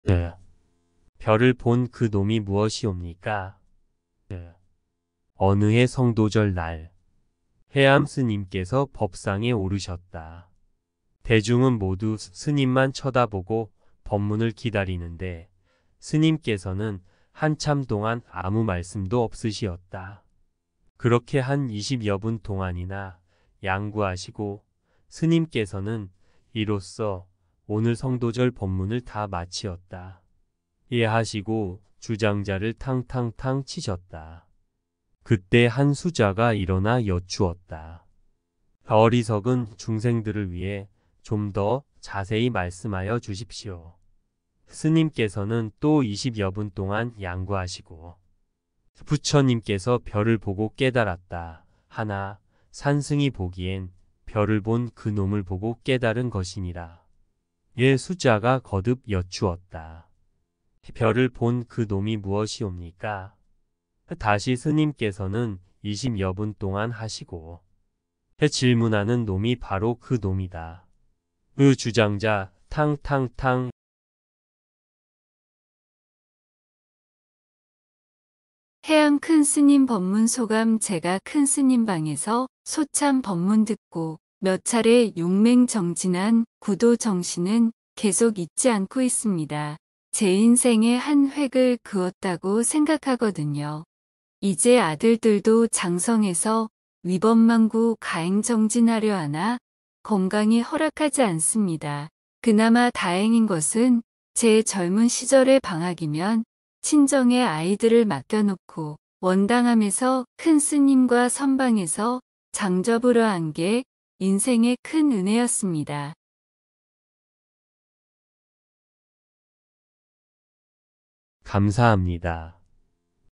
"별을 본 그 놈이 무엇이옵니까?"다시 스님께서는 20여분 동안 良久하시고.. "질문하는 놈이 바로 그 놈이다."혜암스님
별을 본 그 놈이 무엇이옵니까?".. 어느 해 성도절 날.. 혜암스님께서 법상에 오르셨다. 대중은 모두 스님만 쳐다보고 법문을 기다리는데, 스님께서는 한참동안 아무 말씀도 없으시었다. 그렇게 한 20여분 동안이나 양구하시고 스님께서는 "이로써 오늘 성도절 법문을 다 마치었다." 하시고, 주장자를 탕 탕 탕 치셨다. 그때 한 수좌가 일어나 여쭈었다. "어리석은 중생들을 위해 좀 더 자세히 말씀하여 주십시오."스님께서는 또 20여분 동안 양구하시고.. "부처님께서 별을 보고 깨달았다 하나 산승이 보기엔, 별을 본 그 놈을 보고 깨달은 것이니라." 수좌가 거듭 여쭈었다. "별을 본 그 놈이 무엇이옵니까?"다시 스님께서는 20여분 동안 良久하시고.. "질문하는 놈이 바로 그 놈이다." 주장자 탕 탕 탕 ~~~ 혜암큰스님 법문 소감 : 제가 큰스님 방에서 소참 법문 듣고 몇차레 용맹정진한 구도 정신은 계속 잊지 않고 있습니다. 제인생에 한 획을 그었다고 생각하거든요..이제 아들들도 장성해서 위법망구 가행정진 하려 하나 건강이 허락하지 않습니다. 그나마 다행인것은 제 젊은 시절에 방학이면 친정에 아이들을 맡겨 놓고 원당암에서 큰스님과 선방에서 장좌불와한게 인생에 큰 은혜였습니다. 감사합니다.